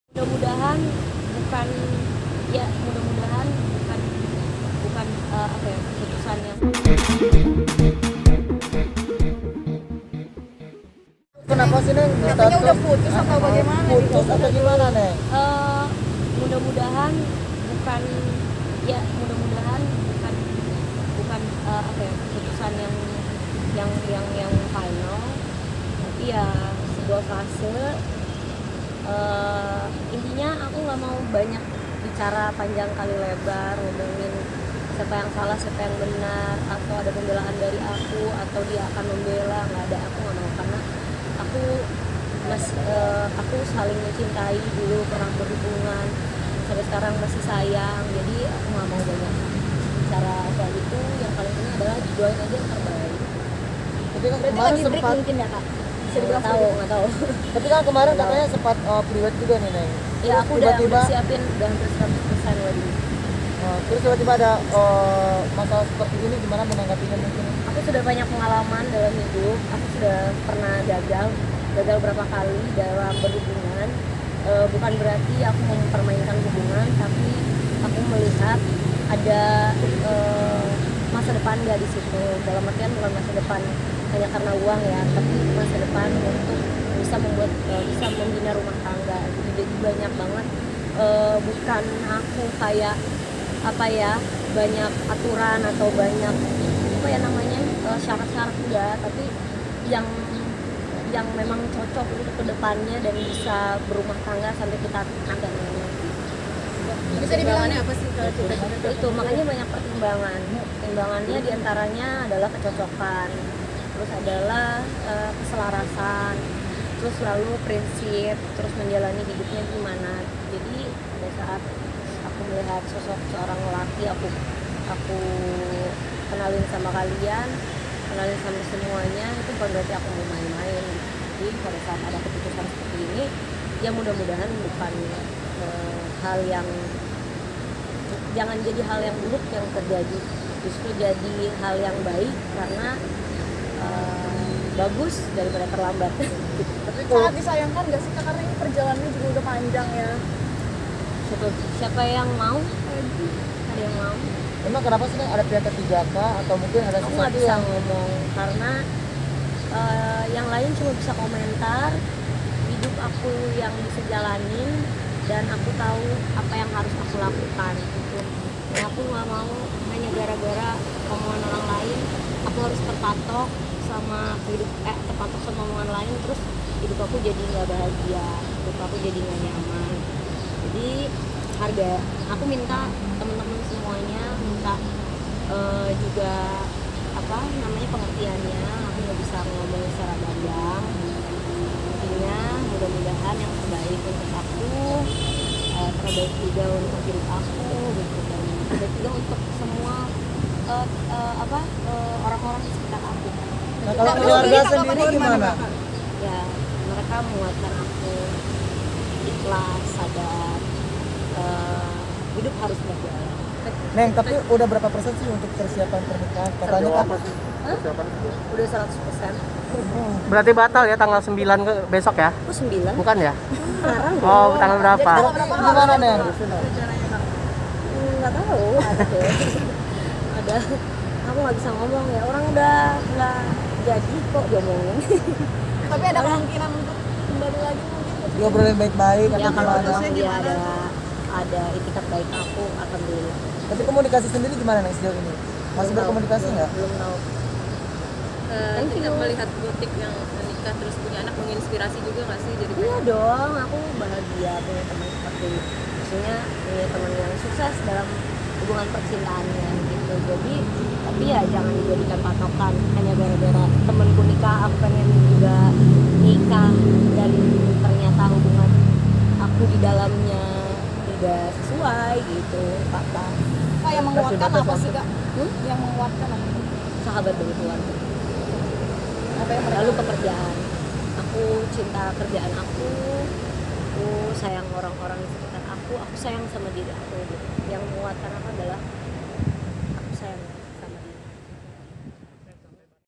CITA CITATA PISAH DARI PACAR DAN BATAL MENIKAH, INI ALASANNYA
Persiapan pernikahan pedangdut Cita Citata sudah rampung 100 persen. Namun tak disangka hal tersebut batal. Cita Citata membatalkan pernikahannya dengan alasan ini.